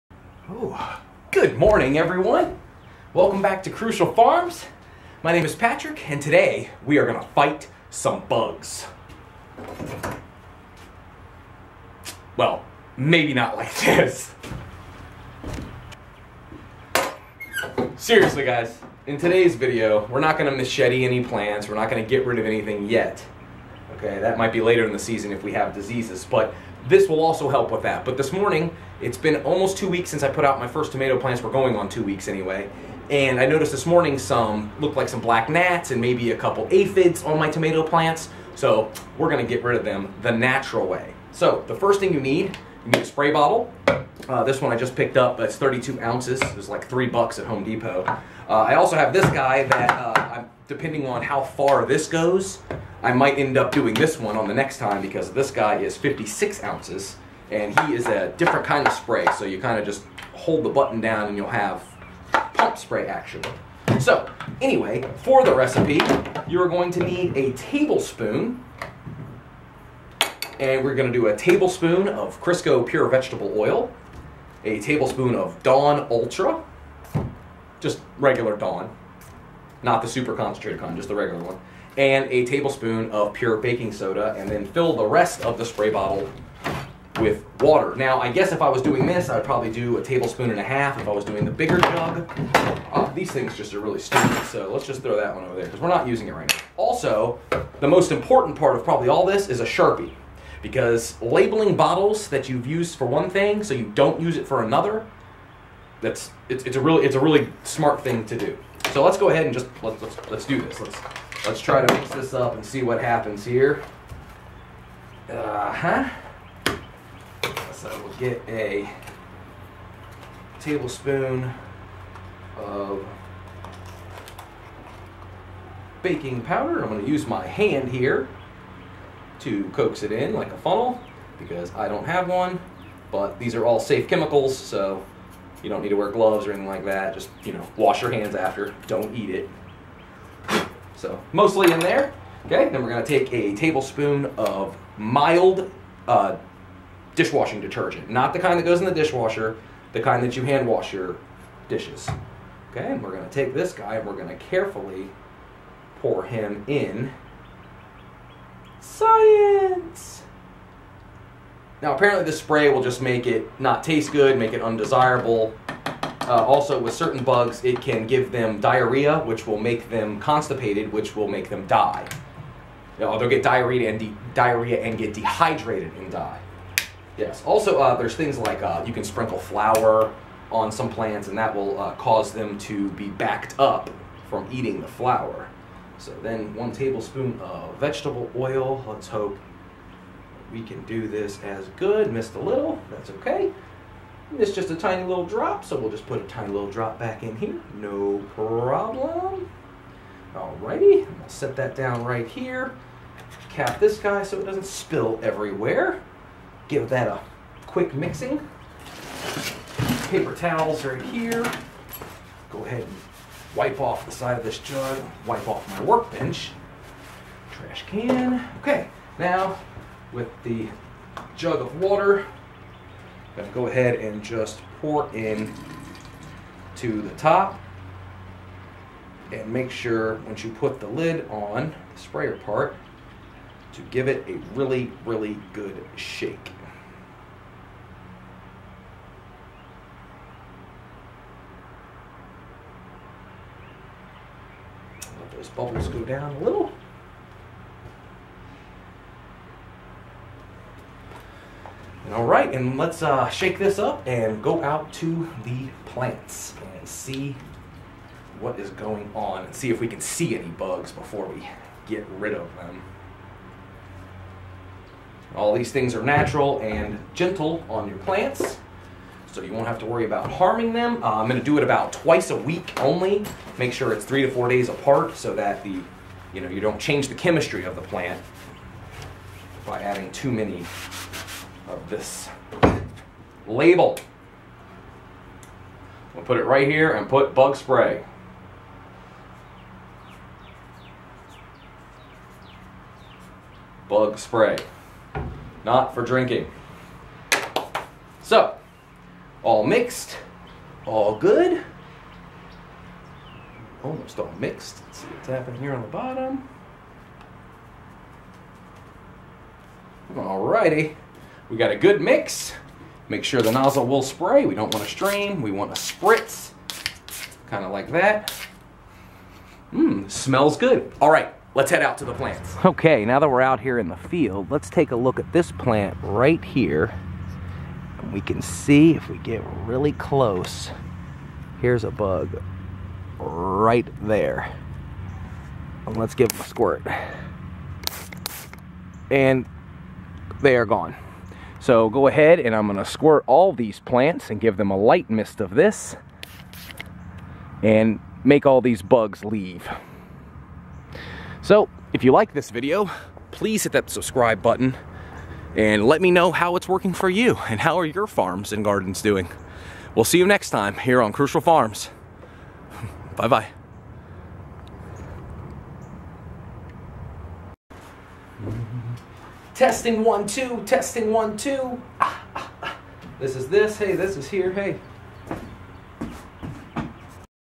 Getting rid of the bugs off my plants
In todays video i show you a natural way to keep the pests at bay. mixing vegetable oil, dish soap and baking soda with water in a spray bottle twice a week should help kep all those pests away.

also please follow use on facebook and instagram by searching for Crucial Farms Urban and Fresh.

Thanks so much and get growing something.